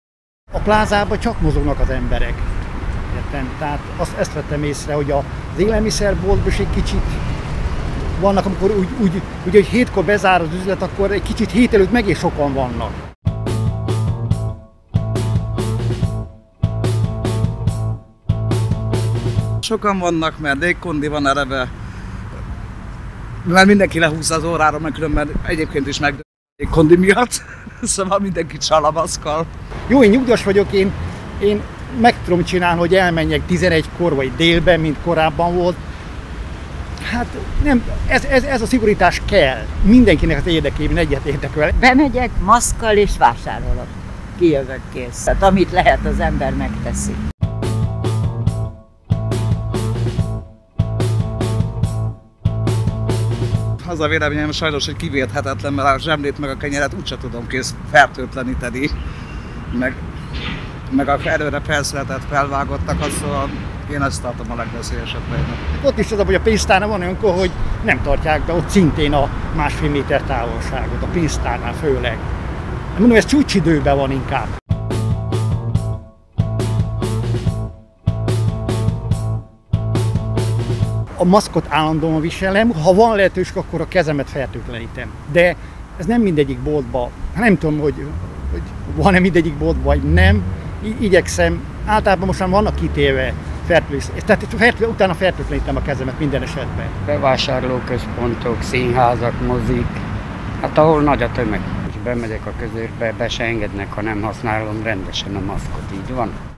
A nap kérdése: Ön szerint is a plázában terjed a legjobban a koronavírus?
Megkérdeztük azt is, hogyan védekeznek a koronavírus ellen.